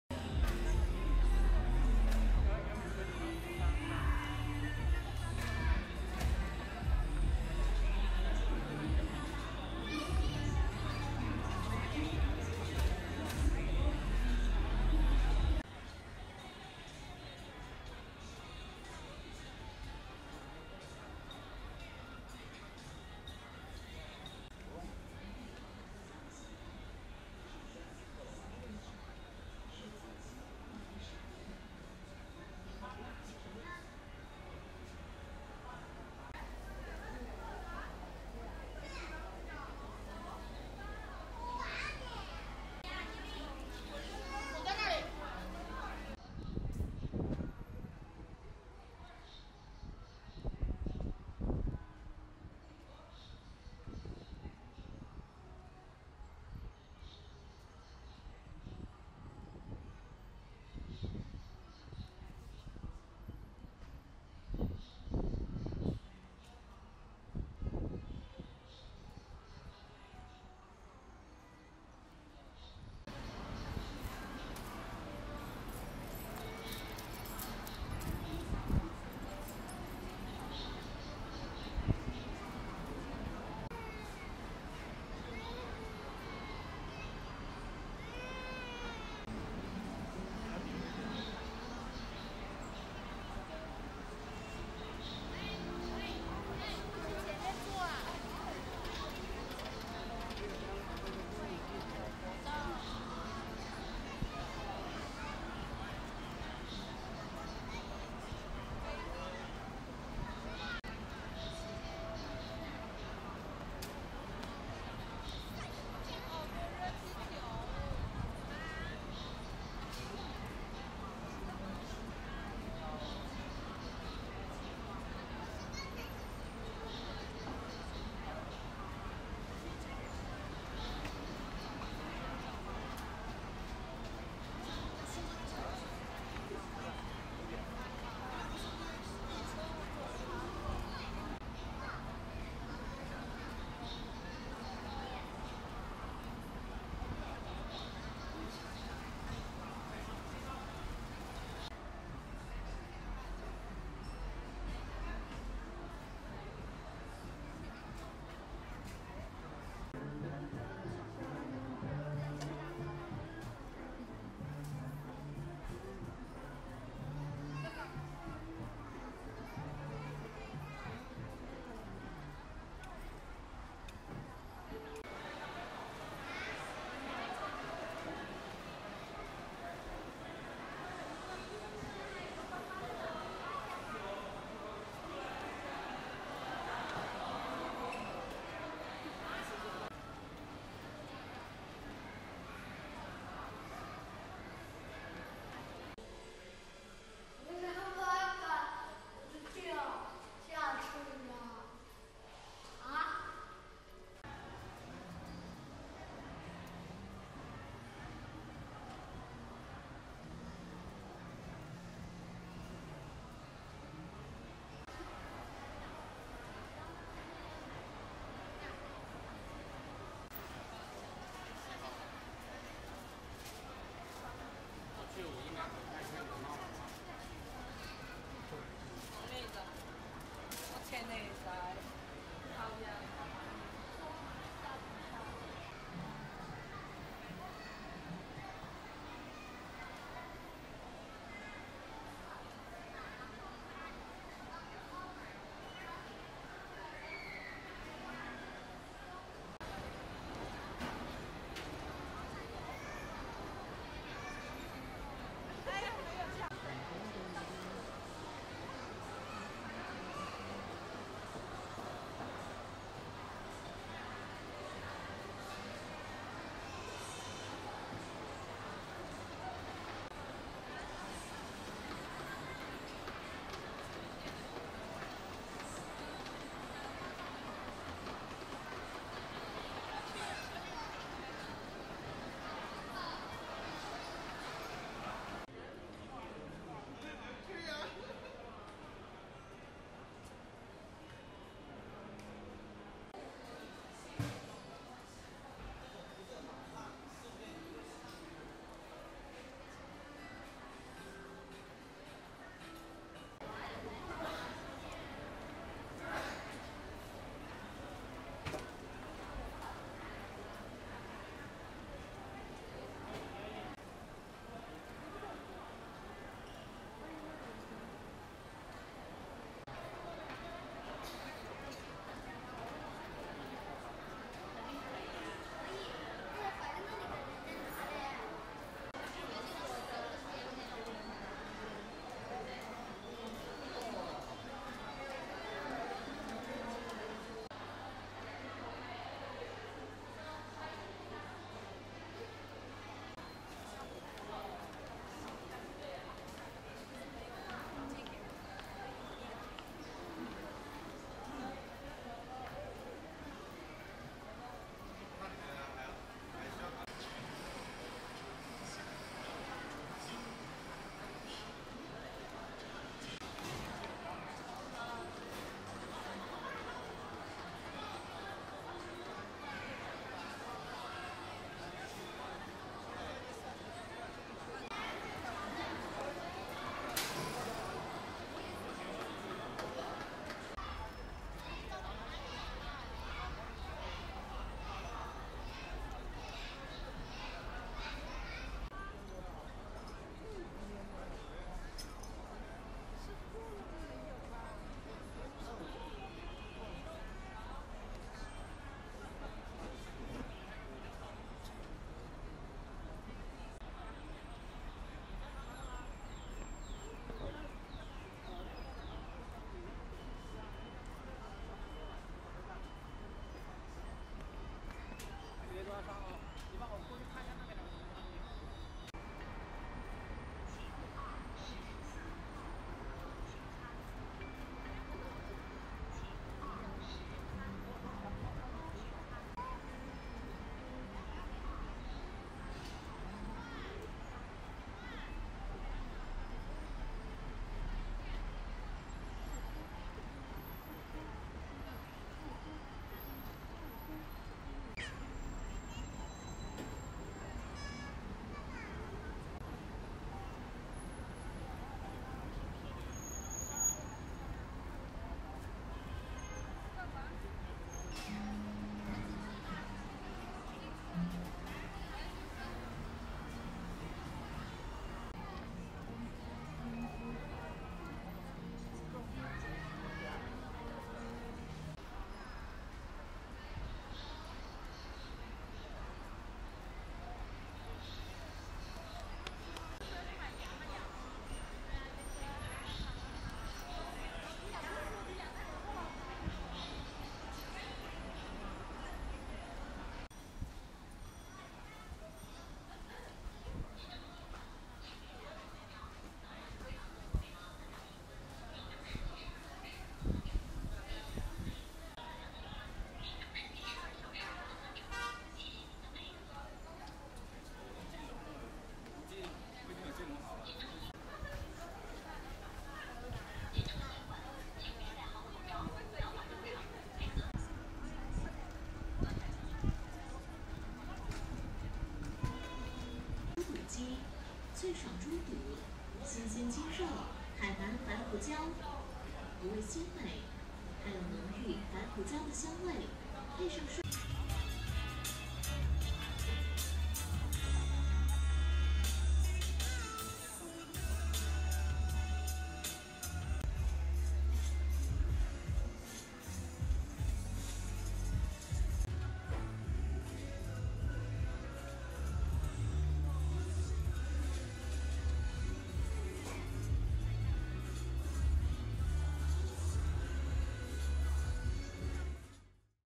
中国街拍：疫情下的高端商场骚杯依然多，还是少妇扭得比较好看！中国の美少女！中国の街並み！Chinese girl！Chinese night street! sexy girl
年轻不知道少妇好！只把少女当个宝！